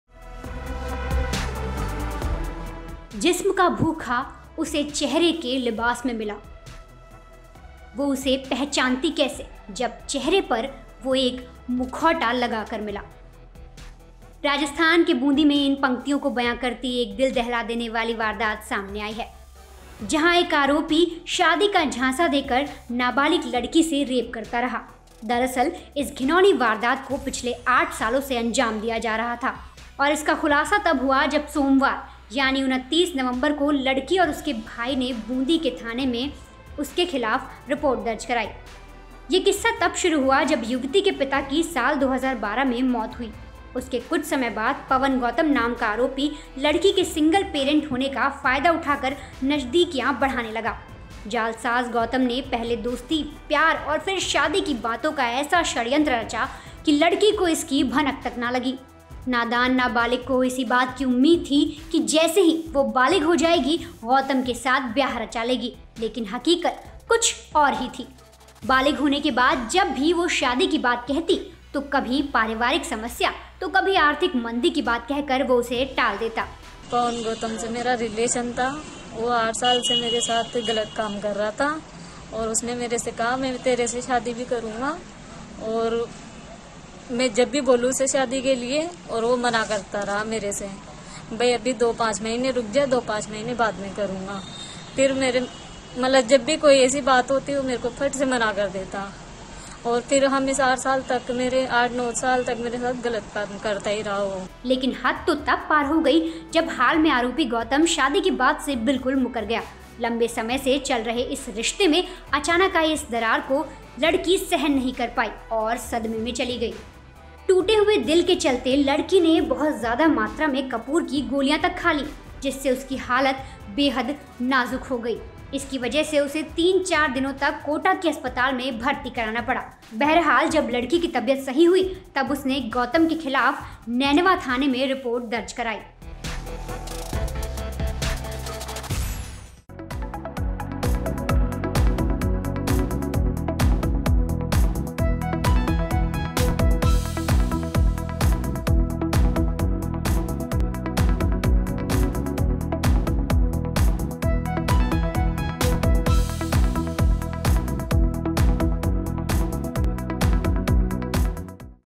एक वादे के भरोसे 8 साल तक ऐसे हुई ये लड़की क़ैद| CRIME TAK
ज़िंदा निर्भया का क्या? CRIME TAK: SAFE INDIA सर्वे में हिस्सा लेने के लिए CLICK करें